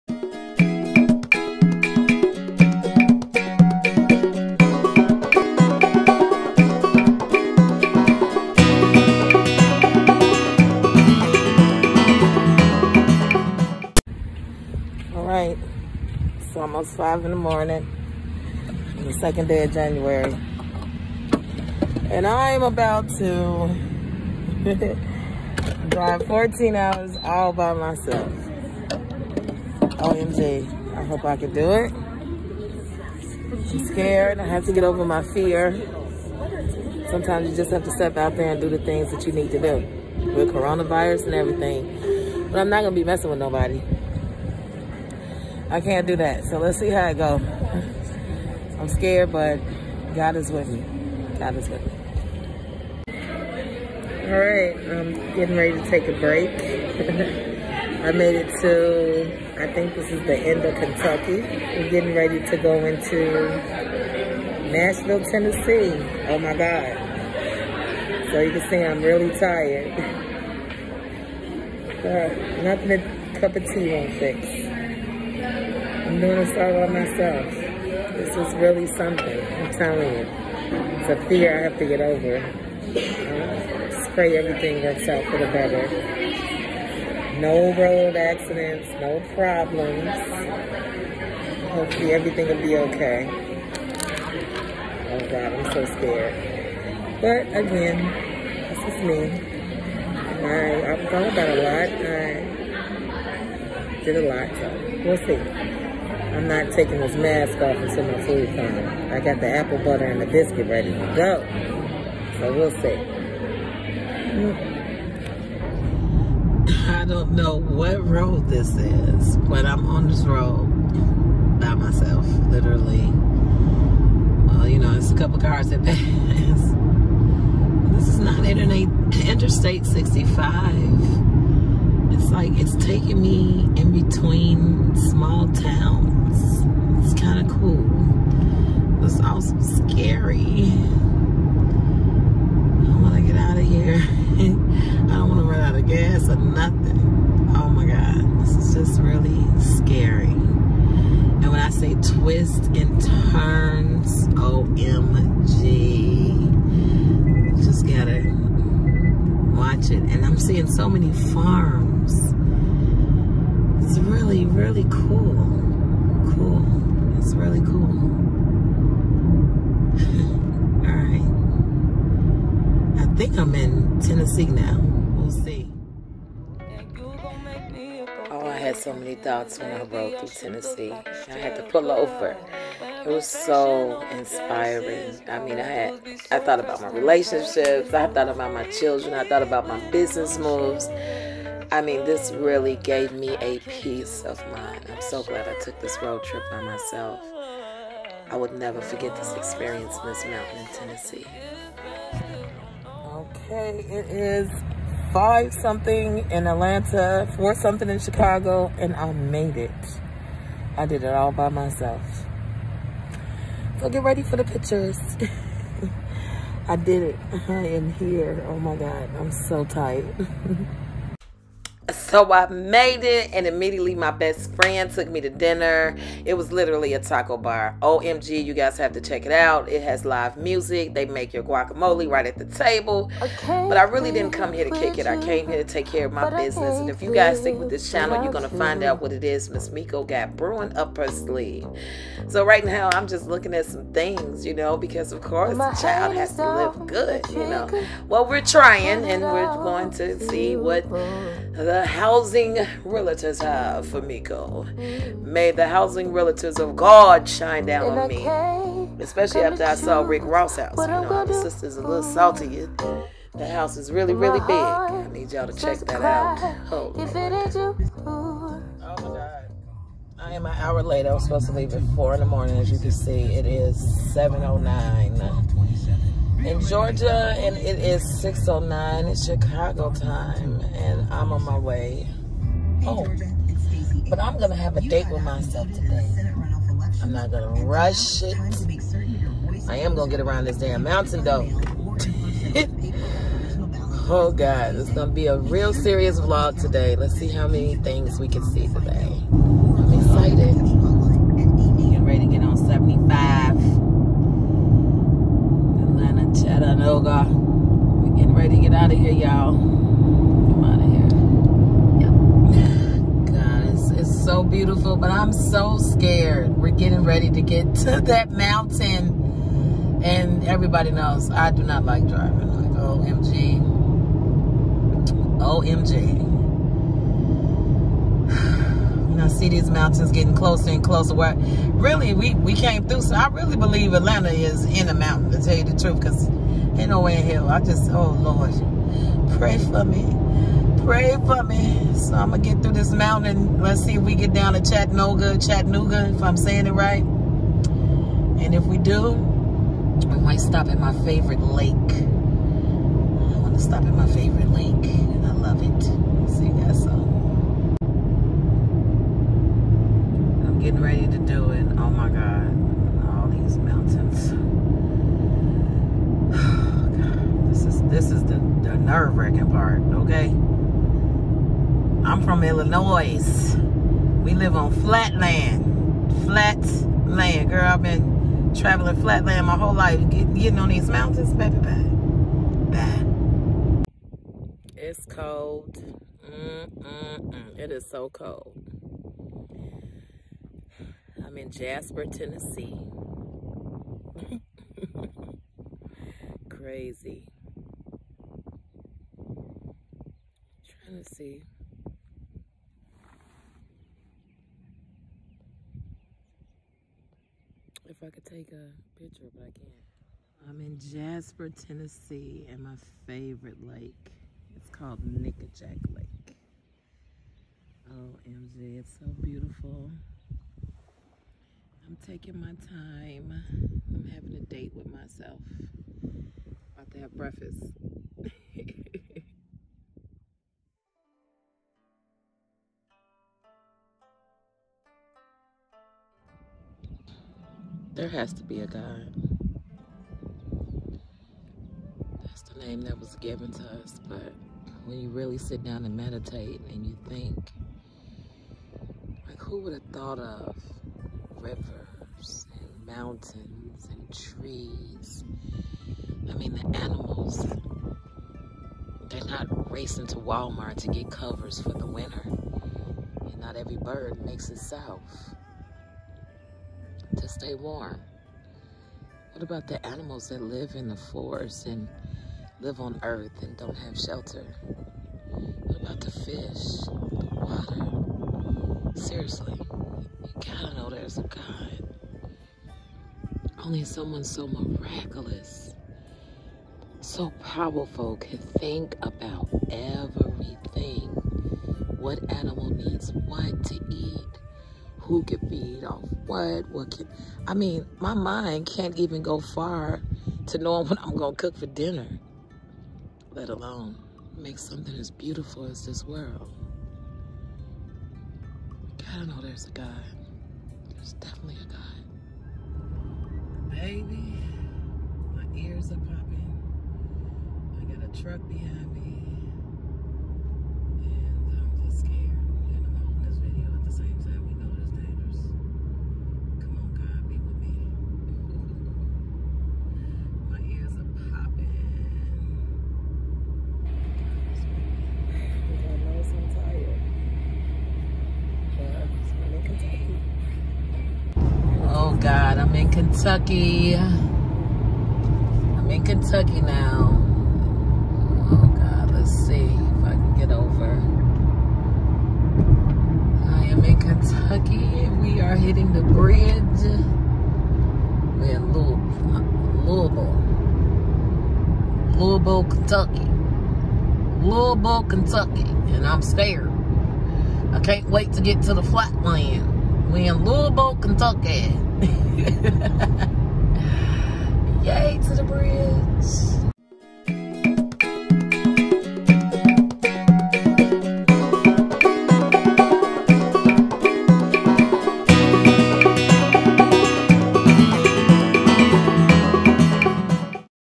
STORYTIME...GEORGIA ROAD TRIP
Amazing sauce! I defeated my fears. I drove to Georgia without anyone with me. Watch the video as I share my experience.